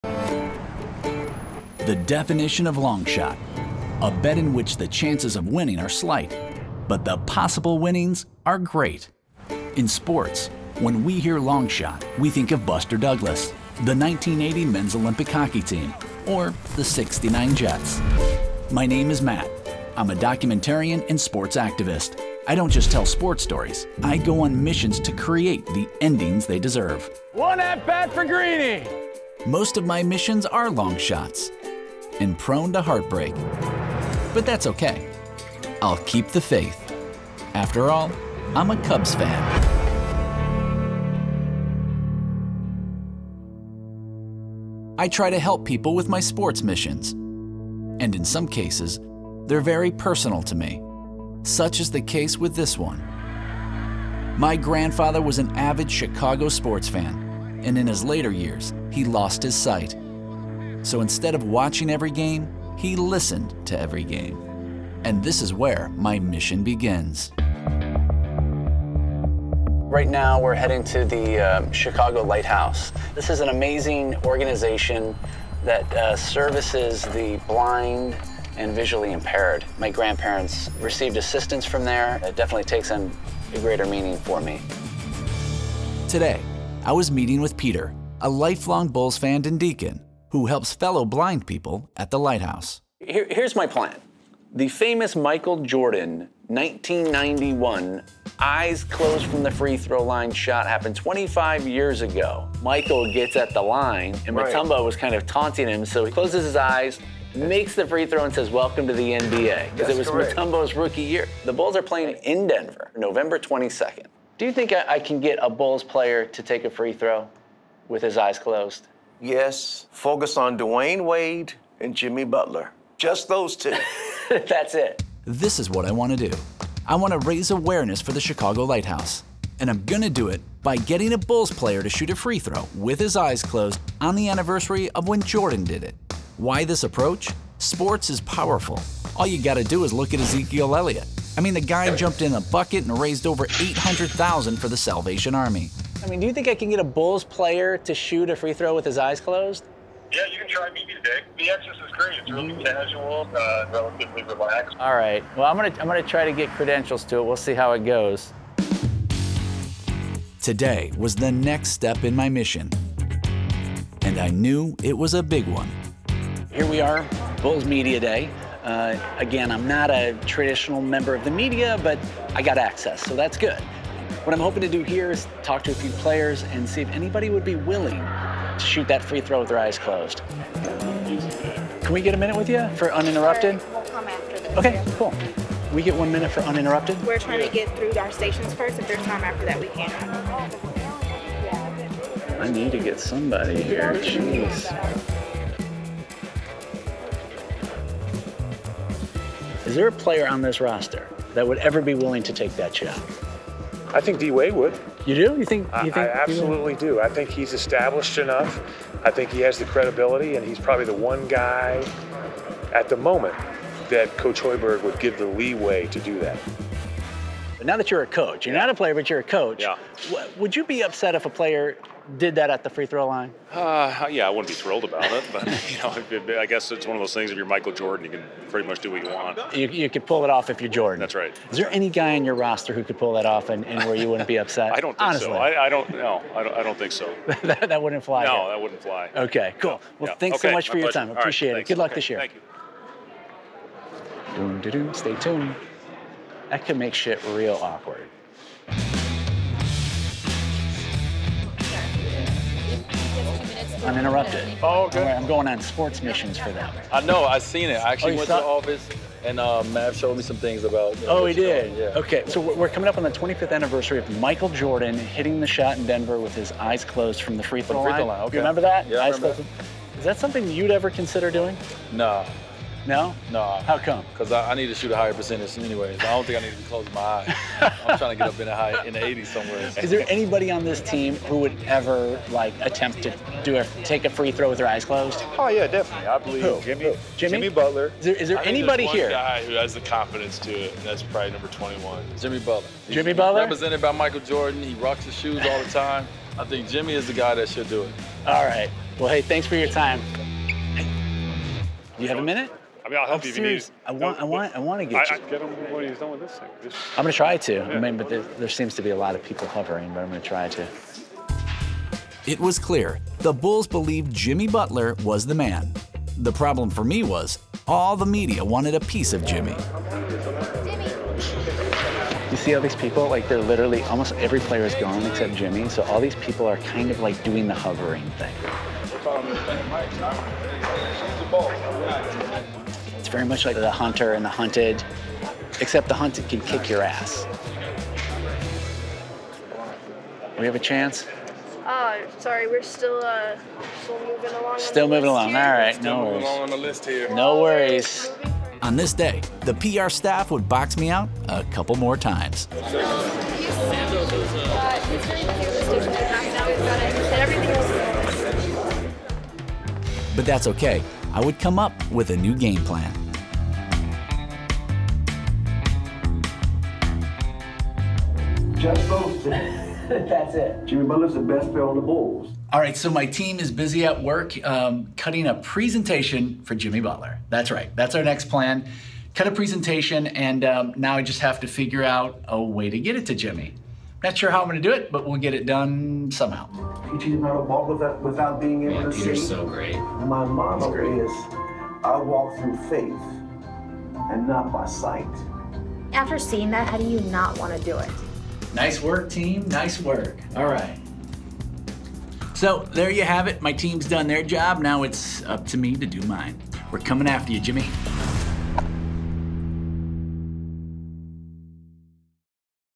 MR. IMPOSSIBLE | Blind Faith [Part 1]
Two decades after Michael Jordan’s famous blind free-throw, sports activist Matt Liston sets out to find a basketball player both to recreate the historic moment, and to raise awareness for the visually-impaired.

Follow Matt Liston as he fights for sports justice and engages the audience to help achieve the impossible.